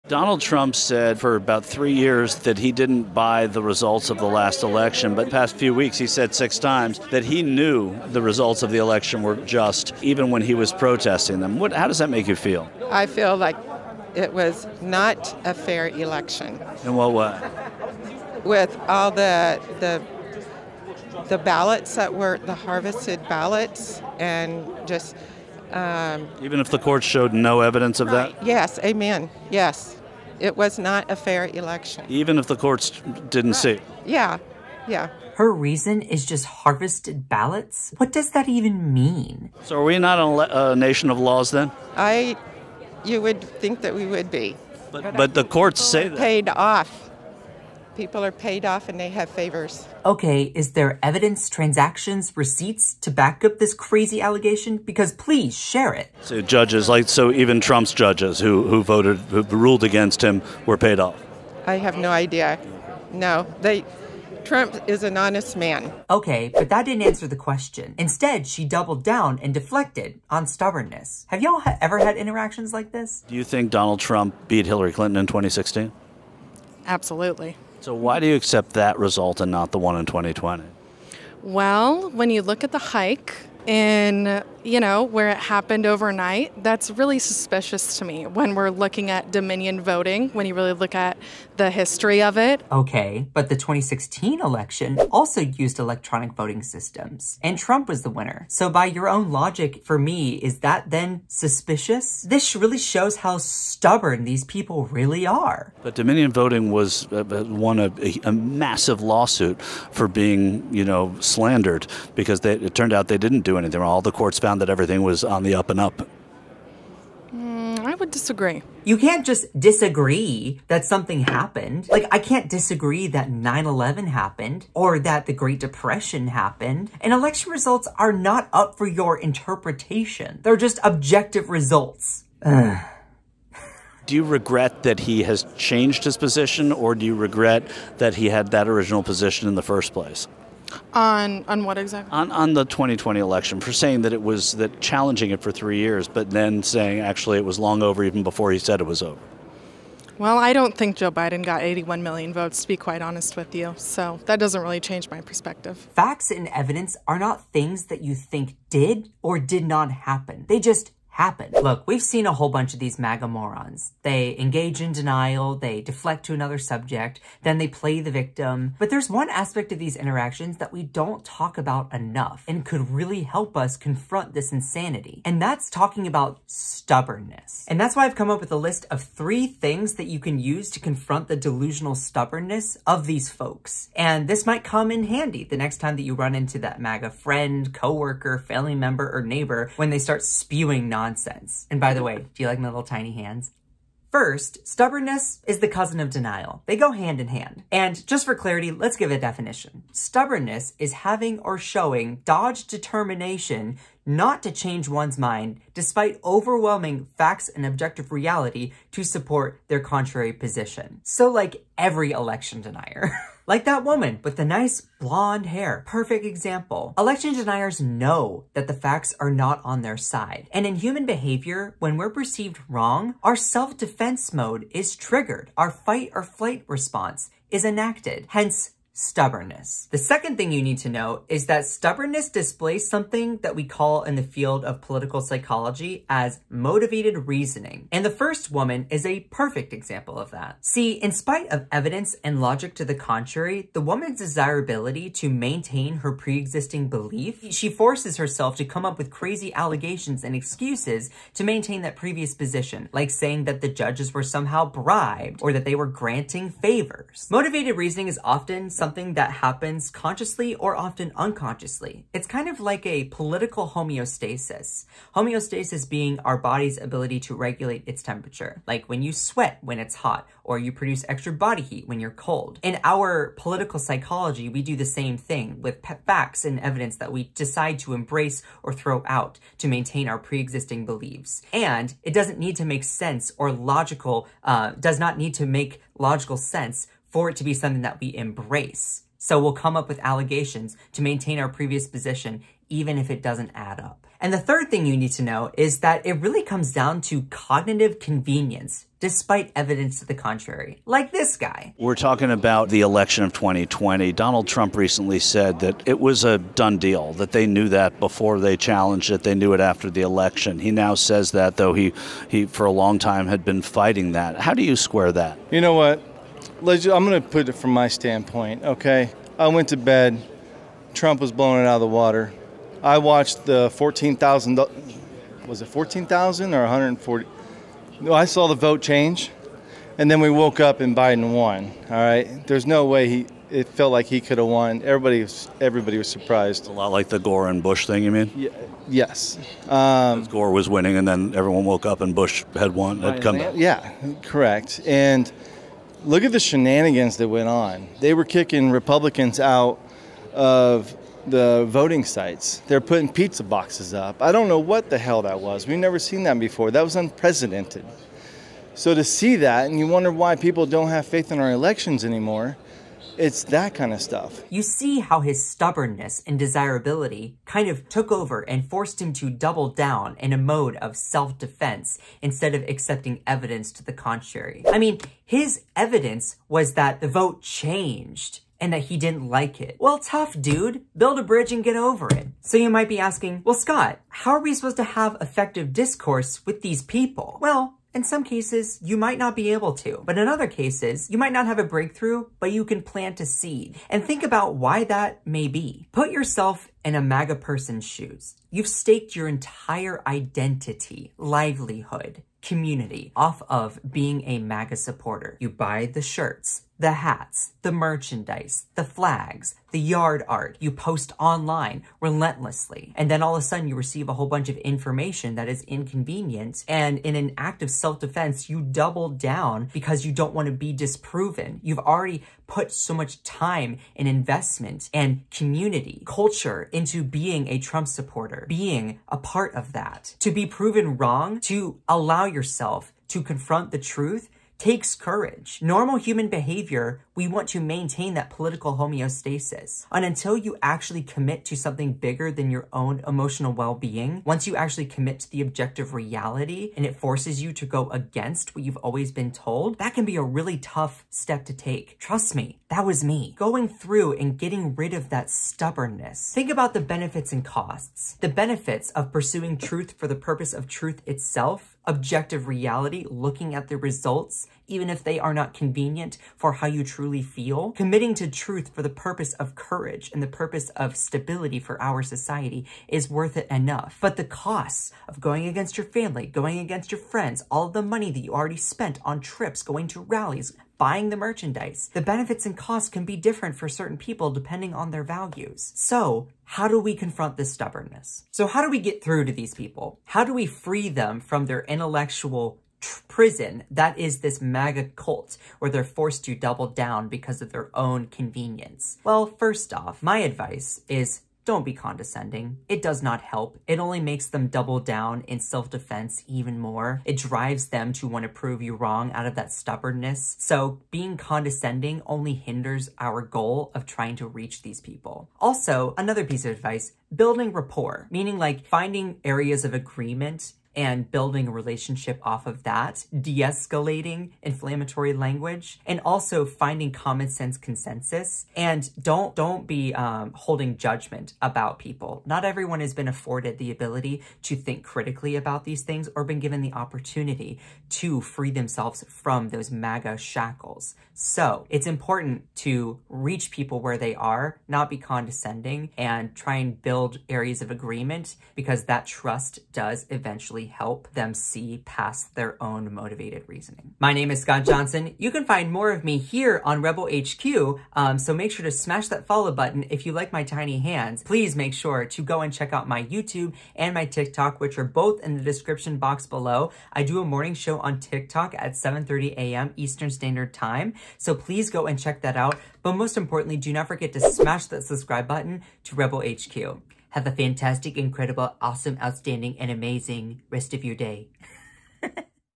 MAGA Lady Malfunctions When Asked About Trump's Many Losses
MAGA cannot handle losing at all. Scott Johnson breaks it down on Rebel HQ.